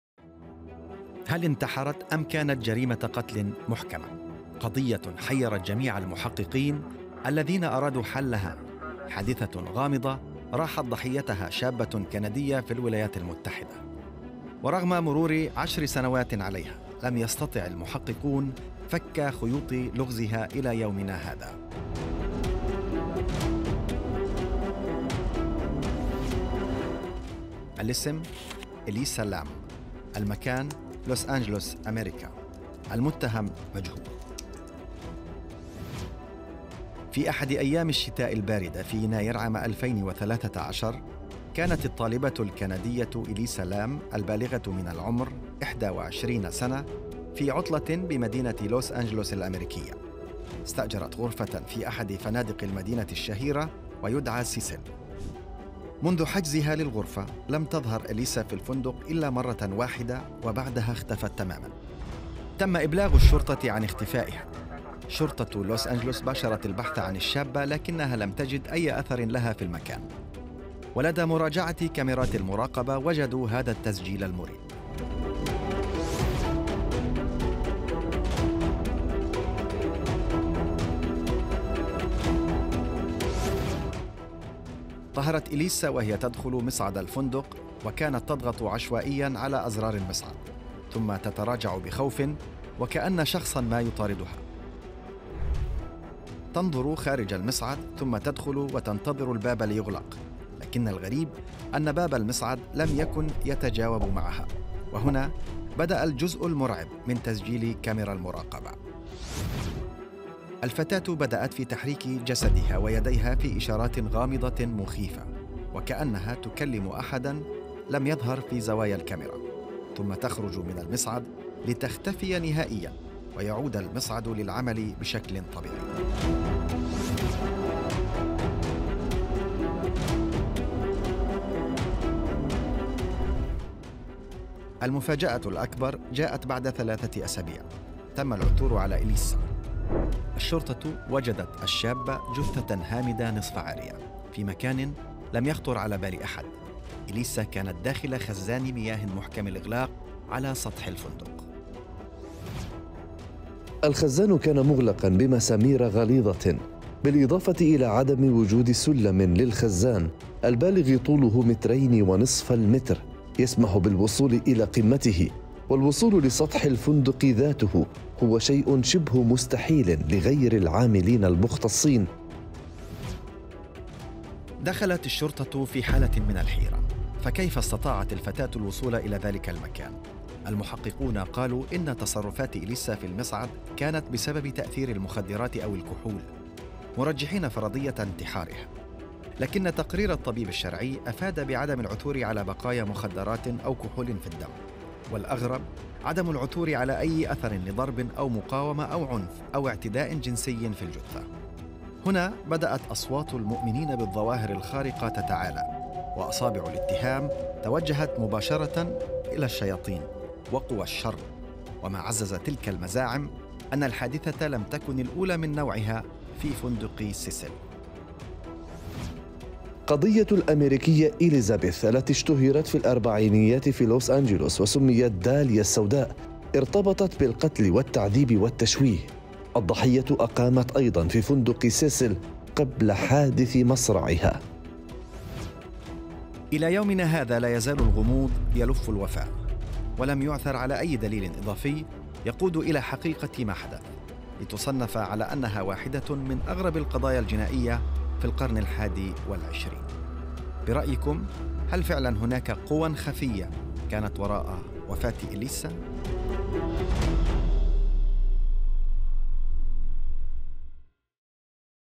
جرائم هزت الكوكب | كيف ماتت؟.. وفاة الشابة إليسا لام.. الحادثة الأكثر غموضا في الولايات المتحدة
جرائم_هزت_الكوكب | هل انتحرت أم قتلت؟.. وفاة الشابة إليسا لام.. 10 سنوات على الحادثة ولم يستطع المحققون فك لغزها إلى يومنا هذا

لمعرفة أحدث المستجدات.. عبر البث المباشر من هنا ⤵
وبإمكانكم متابعة العربية على منصات التواصل